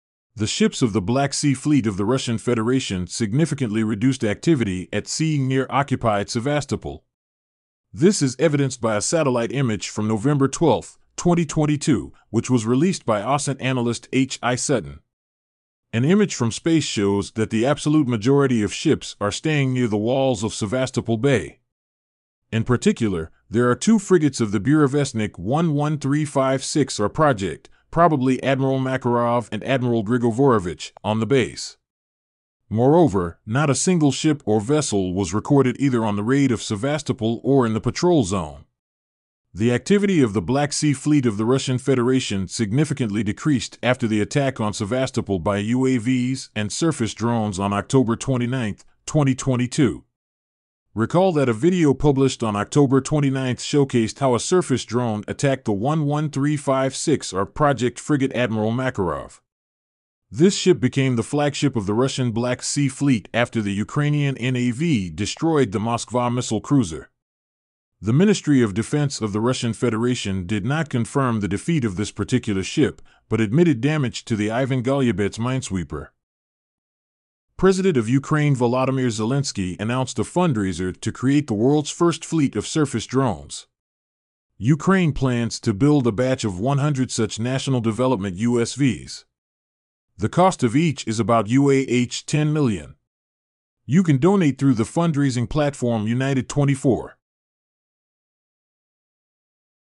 Russian Black Sea Fleet stay in Sevastopol Bay, wary of attack of naval drones
The ships of the Black Sea Fleet of the Russian Federation significantly reduced activity at sea near occupied Sevastopol.
This is evidenced by a satellite image from November 12, 2022, which was released by OSINT analyst H I Sutton.
An image from space shows that the absolute majority of ships are staying near the walls of Sevastopol Bay.
In particular, there are two frigates of the Burevestnik 11356R project (probably Admiral Makarov and Admiral Grigovorovich) on the base.
Moreover, not a single ship or vessel was recorded either on the raid of Sevastopol or in the patrol zone.
The activity of the Black Sea Fleet of the Russian Federation significantly decreased after the attack on Sevastopol by UAVs and surface drones on October 29, 2022.
Recall that a video published on October 29 showcased how a surface drone attacked the 11356R project frigate Admiral Makarov. This ship became the flagship of the Russian Black Sea Fleet after the Ukrainian nav destroyed the Moskva missile cruiser.
According to the Russian Ministry of Defense, nine unmanned aerial vehicles and seven autonomous surface unmanned vehicles were involved in the attack.
President of Ukraine Volodymyr Zelensky announced a fundraiser to create the world’s first fleet of surface drones.
Ukraine plans to build a batch of 100 such national development USVs. The cost of each is about UAH 10 million.